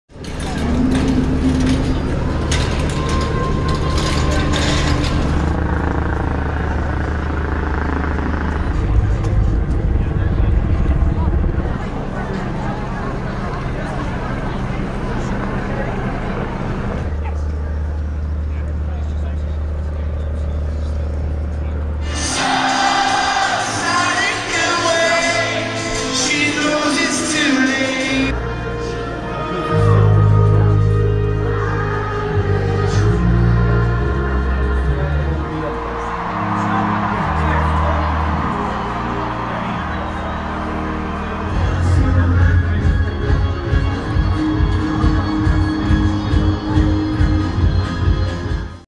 Al concerto "One Love" di Manchester la polizia balla con i fan
Manchester (askanews) - Imponenti misure di sicurezza ma la festa contro la paura è stata un successo. Il grande concerto di Ariana Grande "One Love" a Manchester, 13 giorni dopo l'attentato alla Manchester Arena in cui morirono 22 persone e all'indomani dell'attacco di Londra, in cui hanno perso la vita altre 7 persone, ha visto la partecipazione di circa 50mila persone. E la polizia durante alcuni momenti dello show si è unita alla festa e ha ballato con i giovani in un abbraccio ideale contro ogni paura.